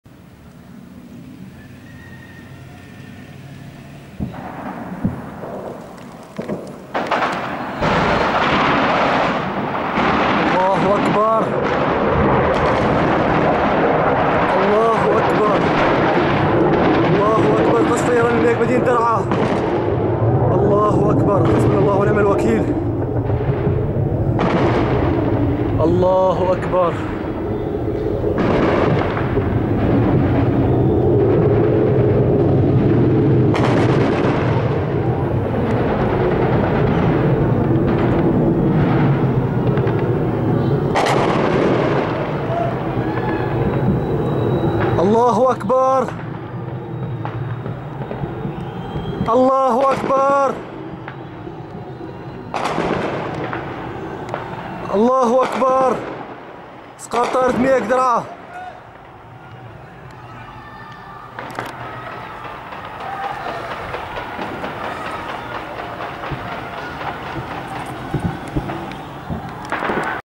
هام : شاهد بوضوح لحظة اسقاط طائرة الميغ أثناء قصفها على احياء درعا البلد
التجمع الاعلامي الموحد بدرعا